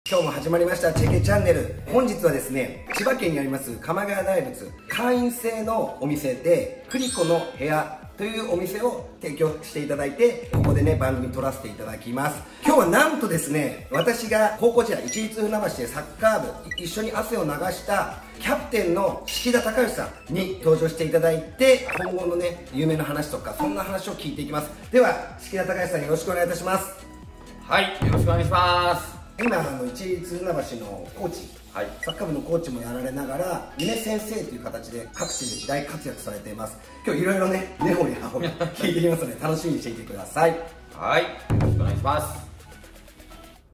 元市立船橋高校サッカー部キャプテン式田高義さんの登場です。
FPとして全国各地で講演を行っております。
2023年8月1日よりYoutubeを始めました！
たくさんの方とコラボしてドキドキワクワク
楽しい企画をつくっていきます。
よろしくお願いいします！

今回は私の市立船橋高校サッカー部時代の同級生でもあり、キャプテンの式田高義さんに来ていただきました。
高校卒業から現在までの経歴を前編・後編と２回にわたってお送りいたします。